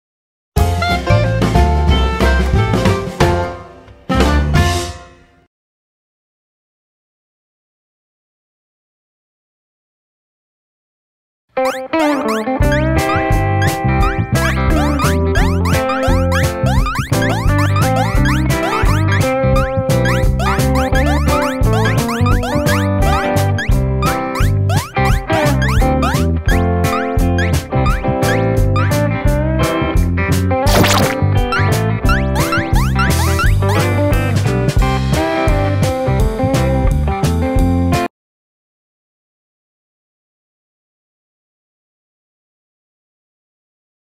【みやざき犬】ススワタリとエンガチョ(201909うめもんじゃ宮崎県産品まつり)
「千と千尋の神隠し」を数日前に見た みやざき犬 むぅ＆かぁ。
もちろん影響されます（笑）
なんと、一番好きな場面は「ススワタリ」が登場する所らしい！